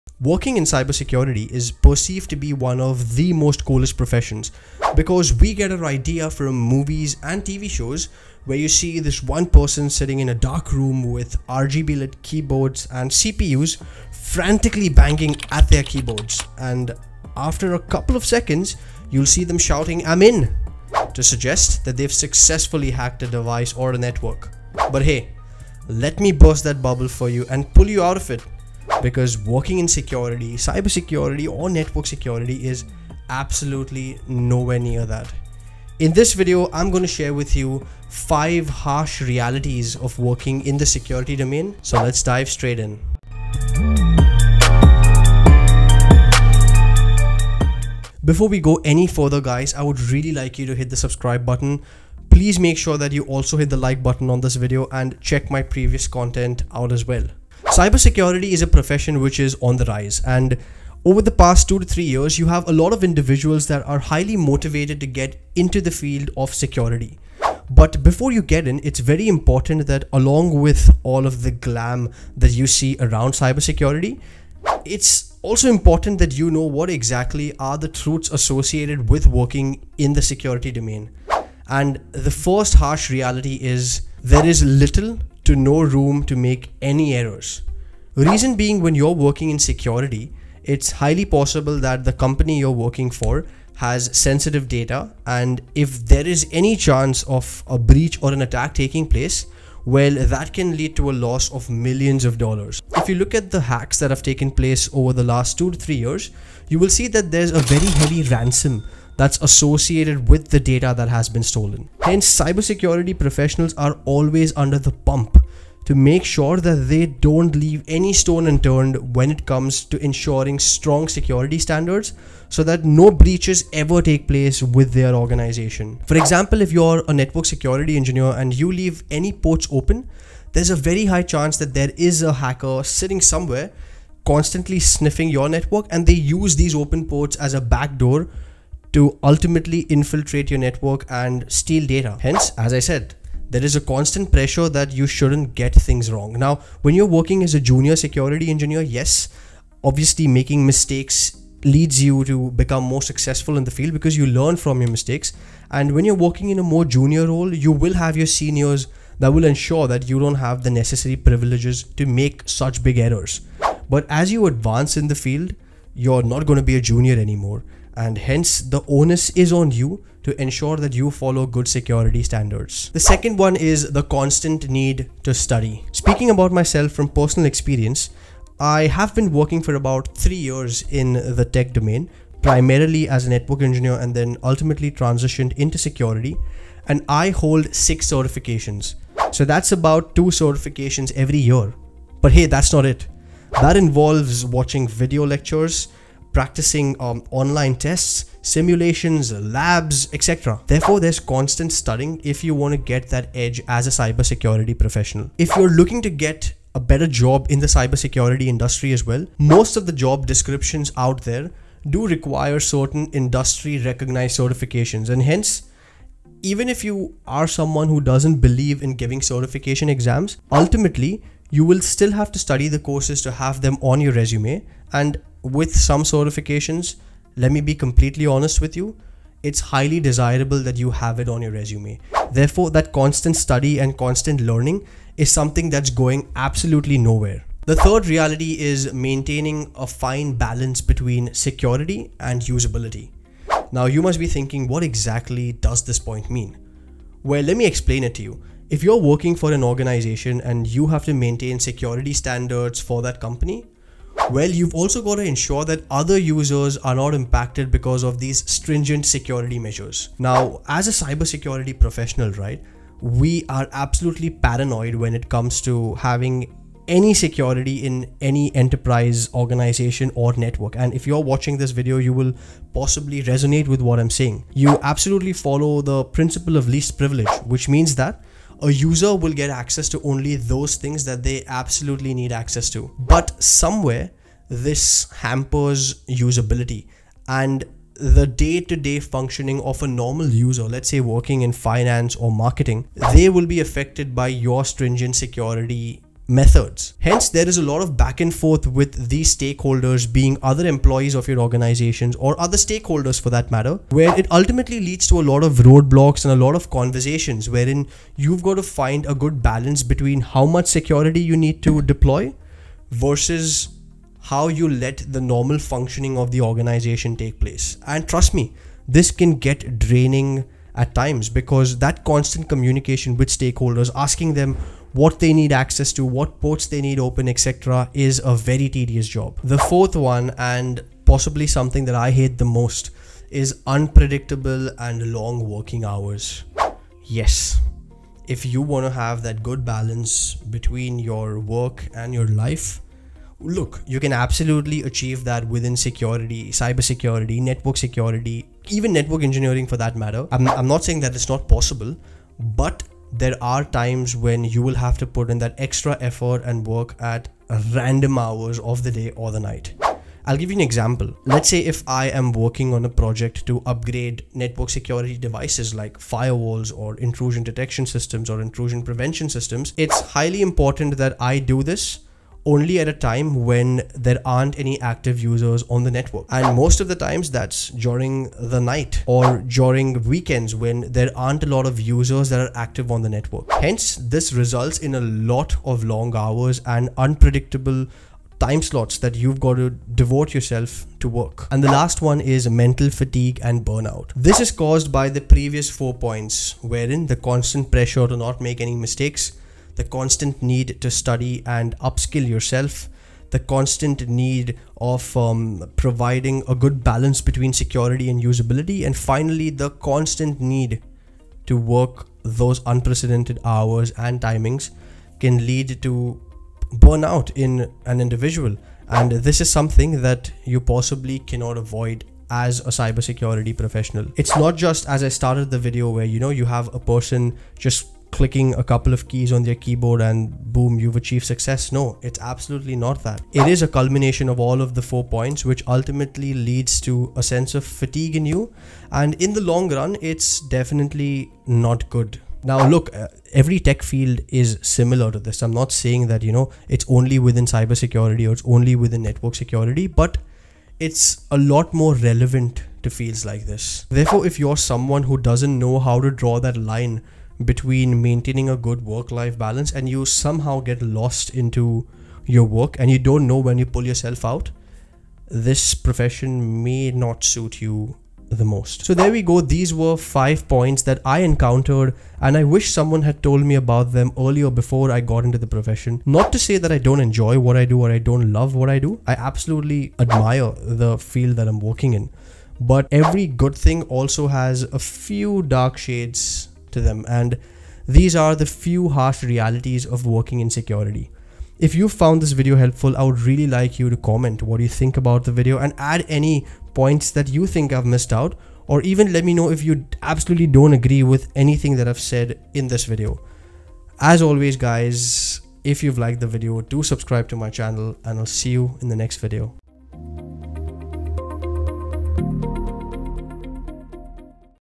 5 Cons of being a Cybersecurity Engineer - Network Security
🚨 Brace yourself as we lift the veil on the lesser-explored side of Network Security Engineering! In this eye-opening video, we're diving deep into the cons that often come with the pursuit of a secure digital landscape. ⚠️

🛑 Timestamps:
00:00 - Introduction
01:22 - No room for errors
02:52 - Constant need to study
03:58 - Balance between security and usability
05:29 - Need to work longer hours than usual
06:31 - Mental fatigue and burnout